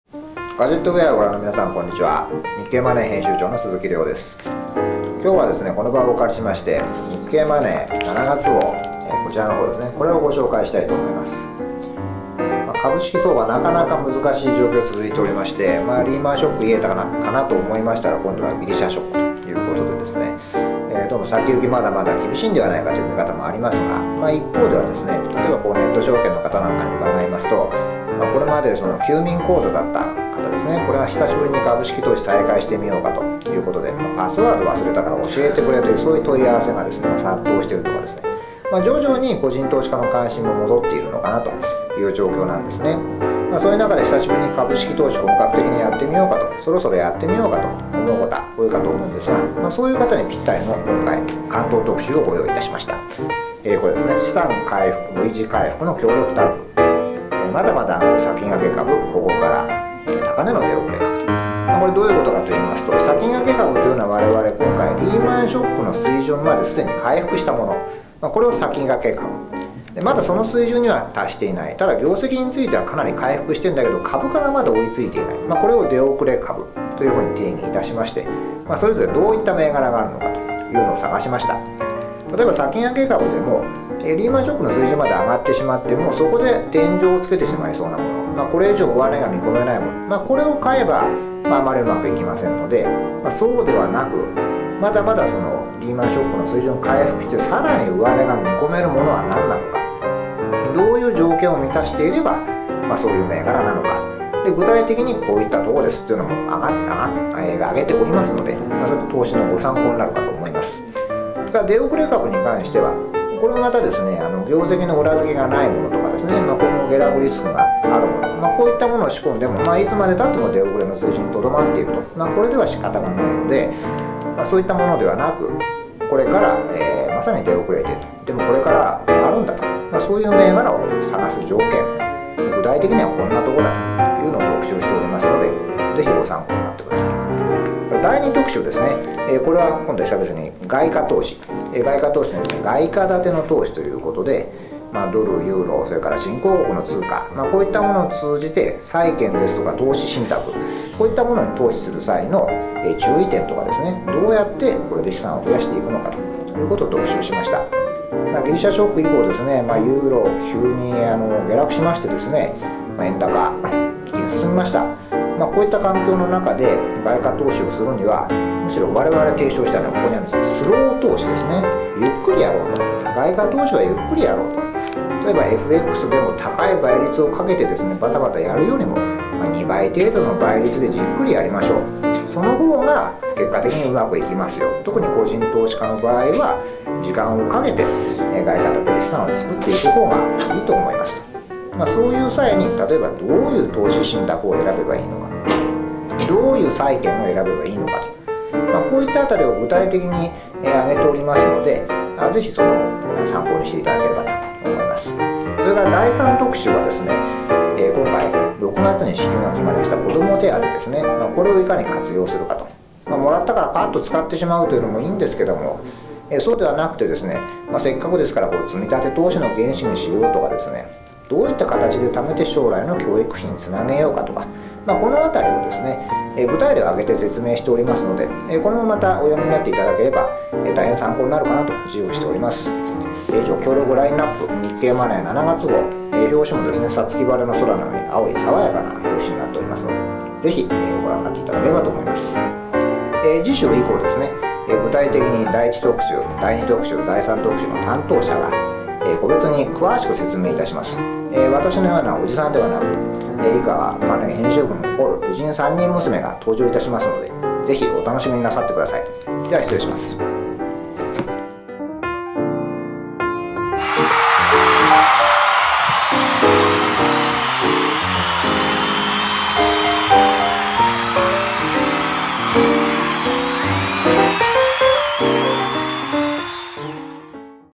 【日経マネー】先駆け株、出遅れ株の動向とは - 第4回
リーマンショック前の水準まで株価が回復した「先駆け株」、業績は良くなっているけれど株価が戻っていない「出遅れ株」にどんな銘柄があるのかを探しました。